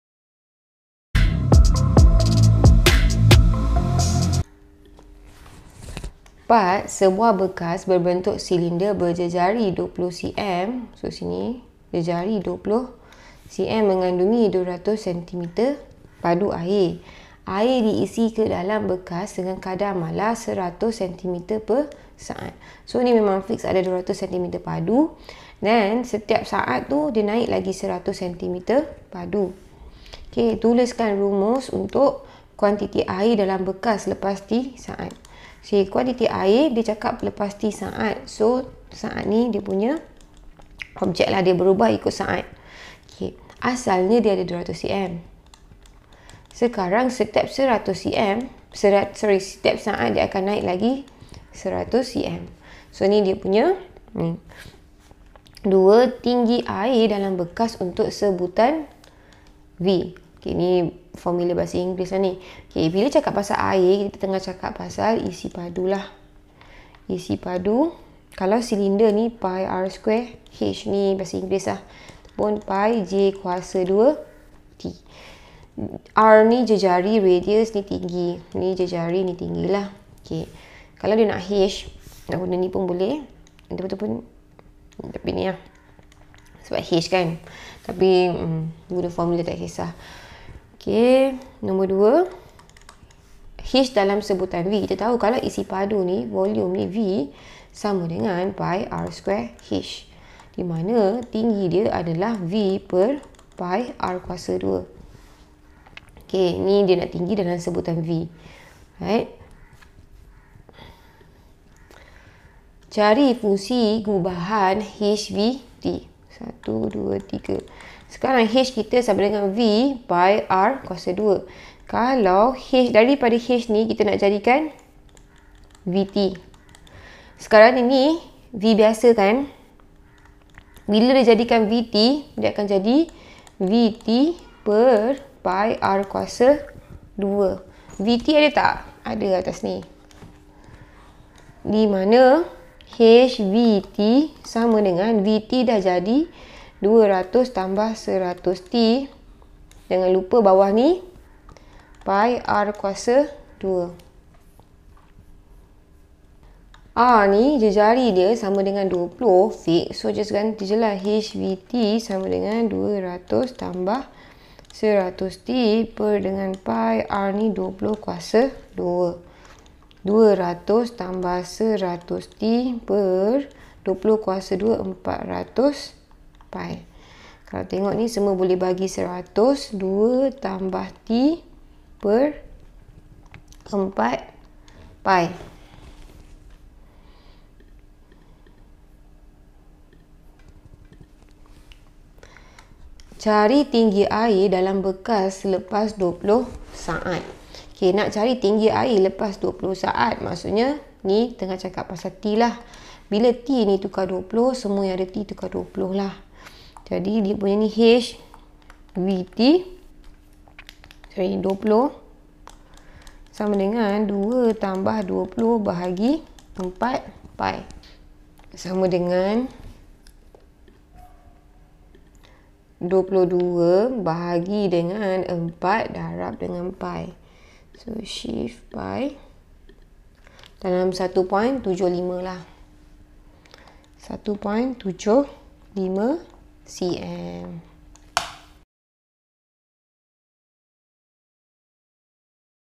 Latih diri 1.7 Soalan 4 | 1.2 Fungsi Gubahan | Bab 1 Fungsi Matematik Tambahan Tingkatan 4
(a) Tuliskan rumus untuk
(i) kuantiti air, v, di dalam bekas itu selepas t saat,
(ii) tinggi air, h, di dalam bekas itu dalam sebutan v,
(iii) fungsi gubahan hv(t).
(b) Cari tinggi air di dalam bekas itu selepas 20 saat.